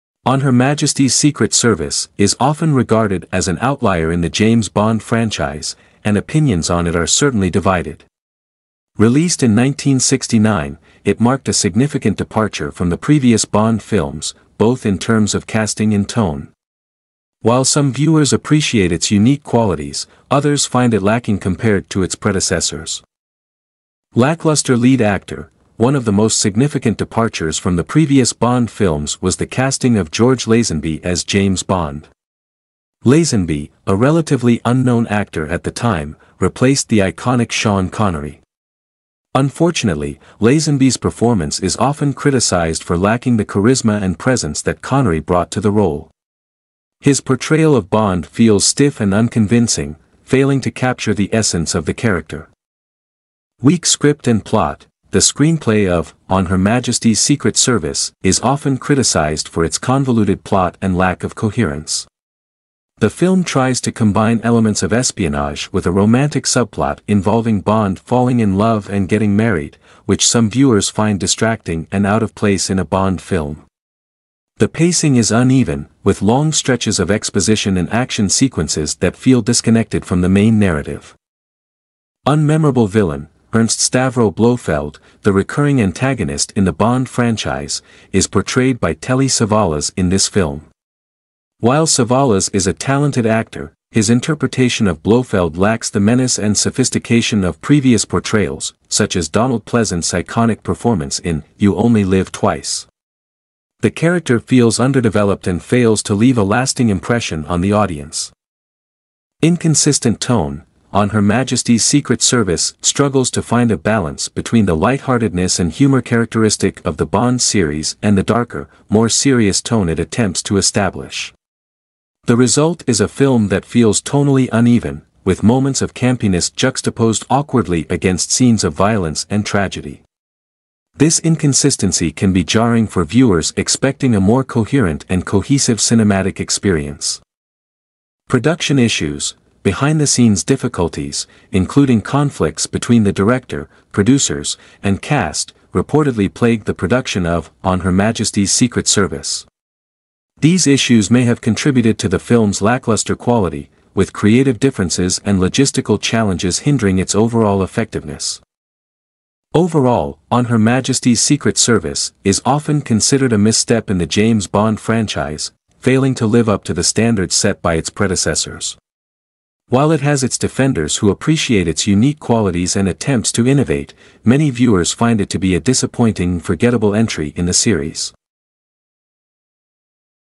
On Her Majesty's Secret Service (1969): A Bond Misfire
In this detailed review, we dissect why "On Her Majesty's Secret Service" is often regarded as a misfire in the iconic James Bond franchise. From George Lazenby's lackluster portrayal of Bond to the convoluted plot and underwhelming villain, we delve into the elements that contribute to its status as a disappointing entry.